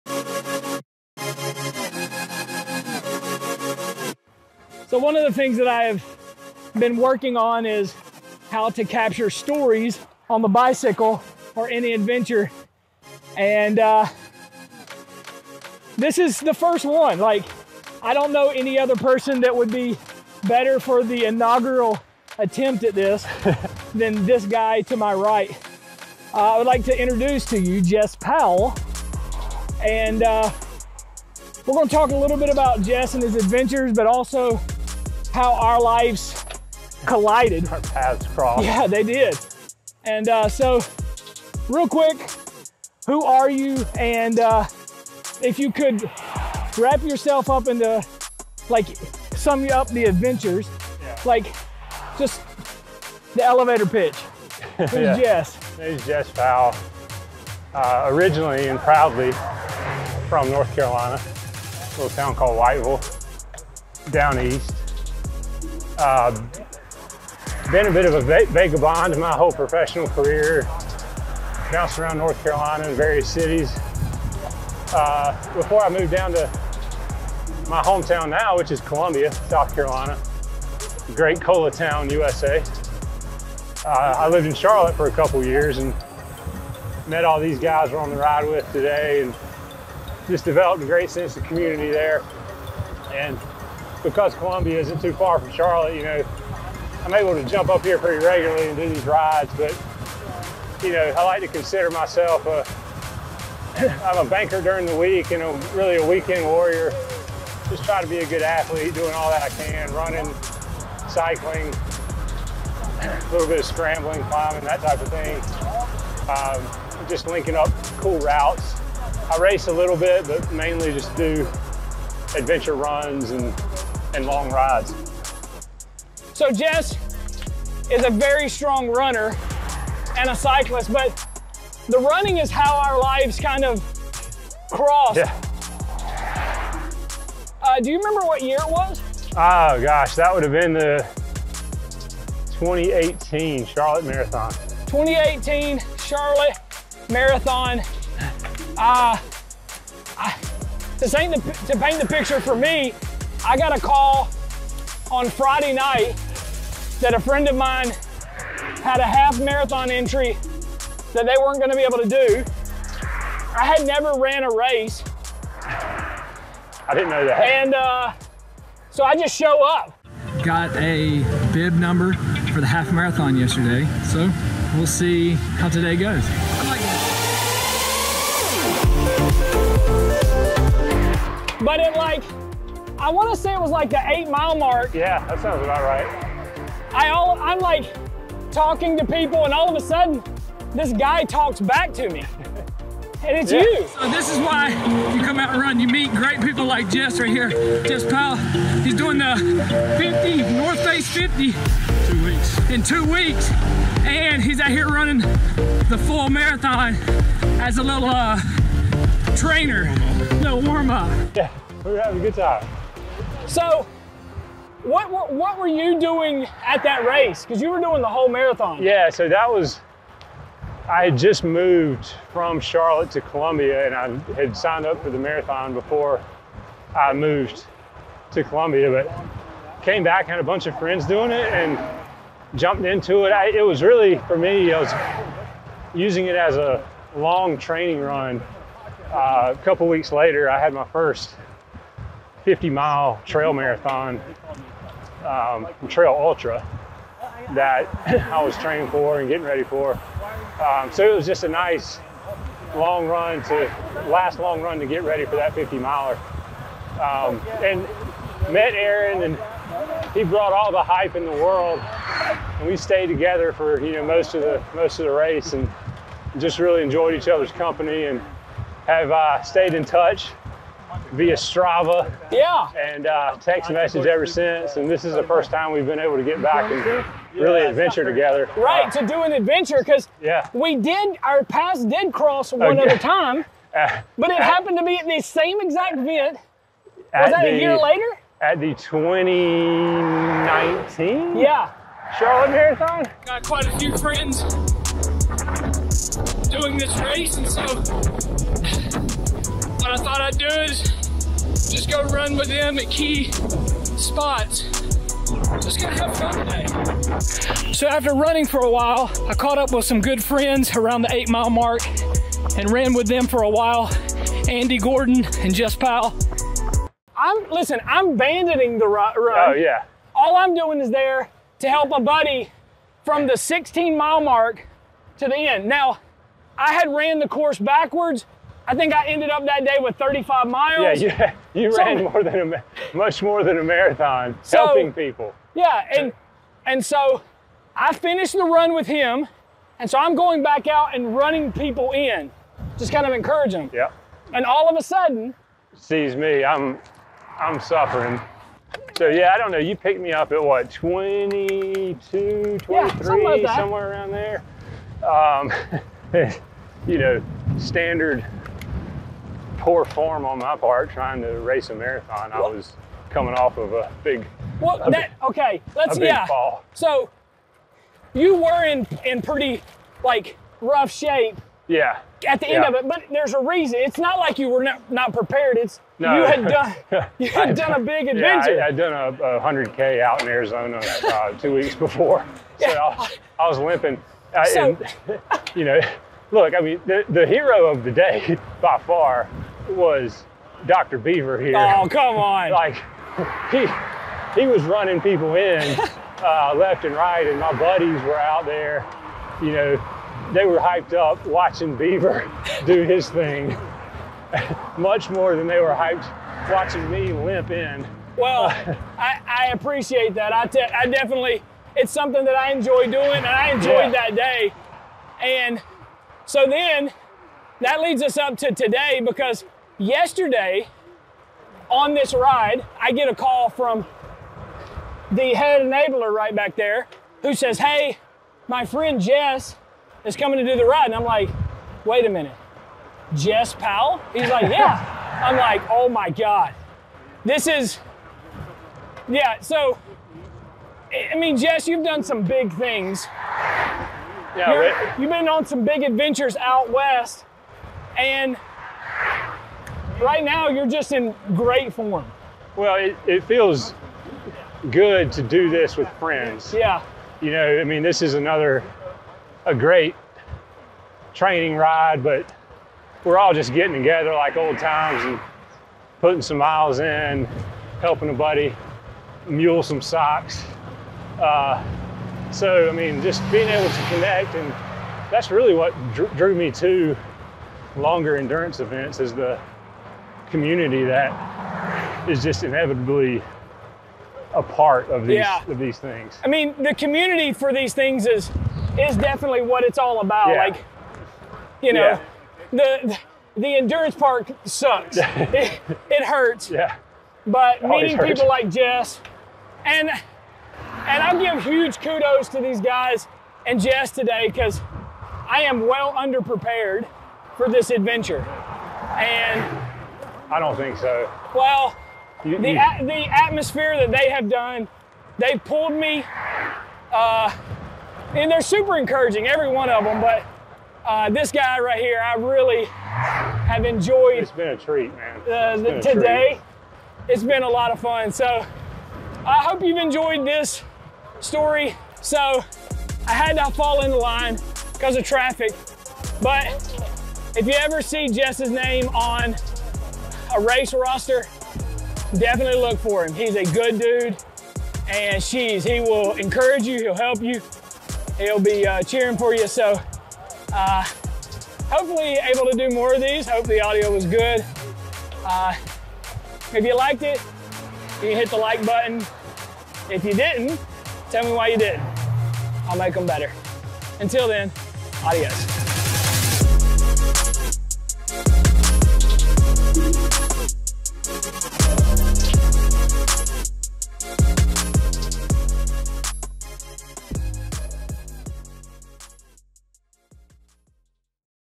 From the Ride | Jess Powell
We at The Suffer Club love sharing stories, Stories of all levels, and in this video, Aaron Beaver chats with Jess Powell.
We find out how they met and hear about some of Jess Powell's crazy adventures.